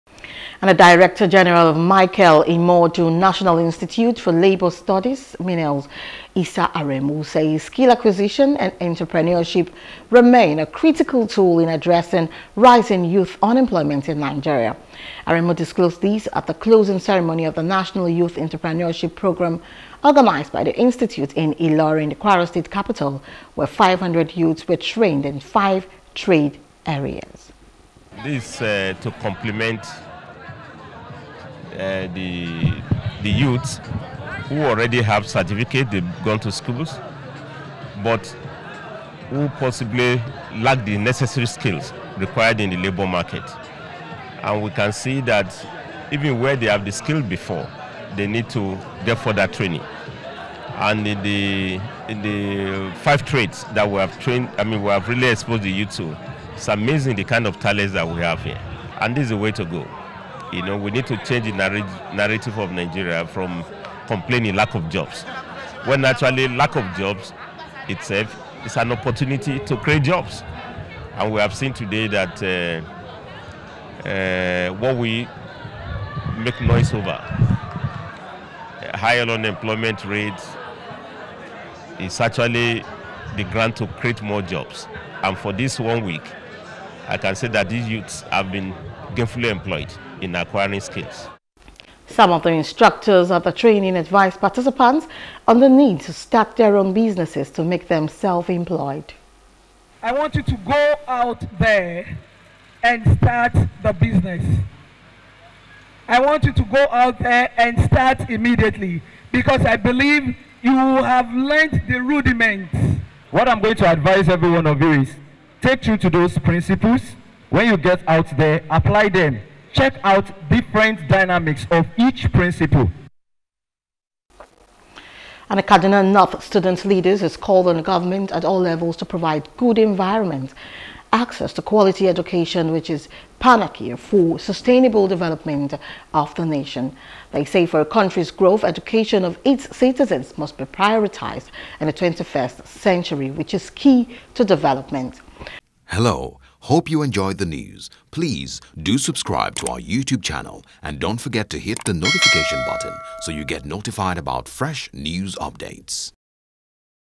Youth Unemployment: Skills Acquisition Critical To Tackle Challenge
The Director General of Michael Imoudu National Institute for Labour Studies (MINILS), Issa Aremu says skill acquisition and entrepreneurship remain a critical tool in addressing rising youth unemployment in Nigeria.

Aremu disclosed this at the closing ceremony of the National Youth Entrepreneurship Programme organized by the Institute in Ilorin the Kwara State capital where 500 youths were trained in five trade areas.